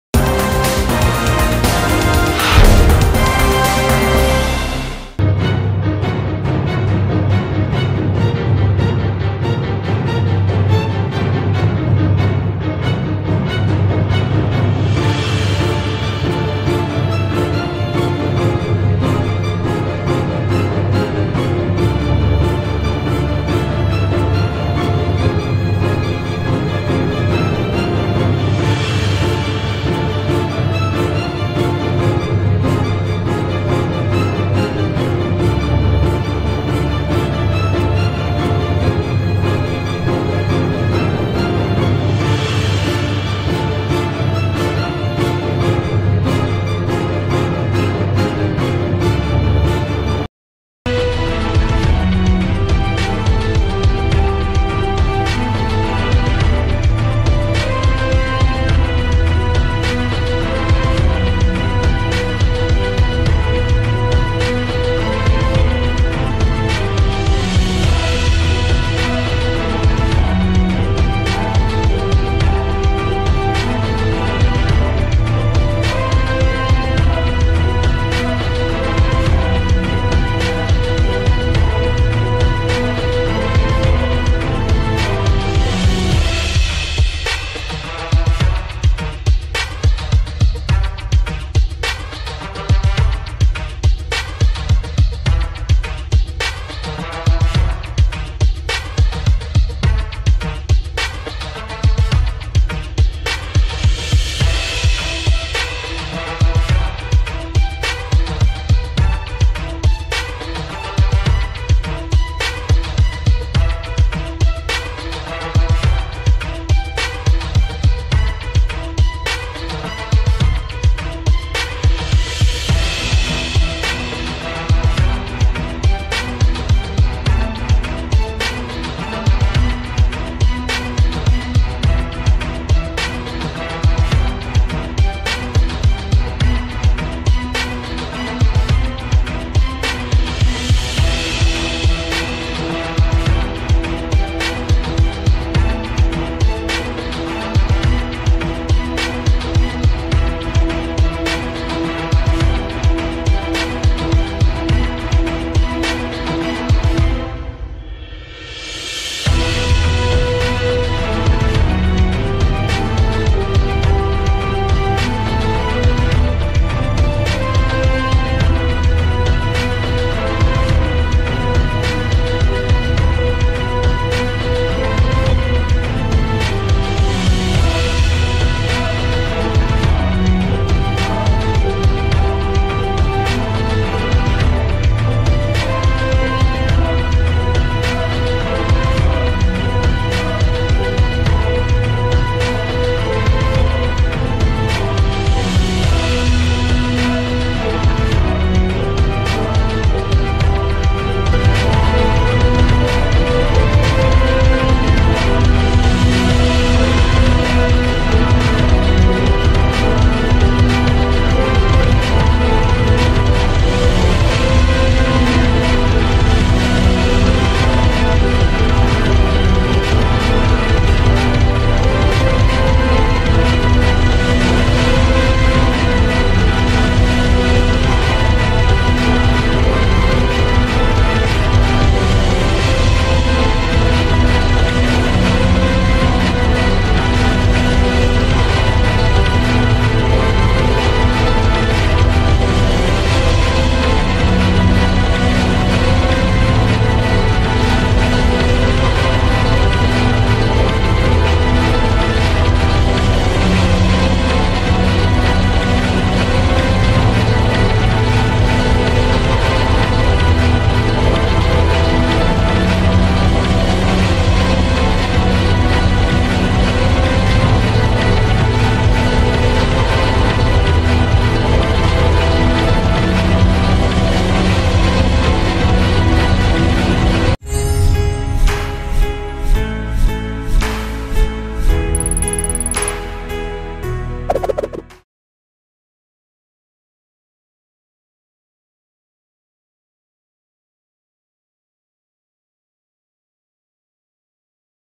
Telangana CM KCR Starts From Begumpet Airport For YS Jagan Swearing-In Ceremony | Mango News
Telangana CM KCR Starts From Begumpet Airport to attend YS Jagan Swearing-In Ceremony.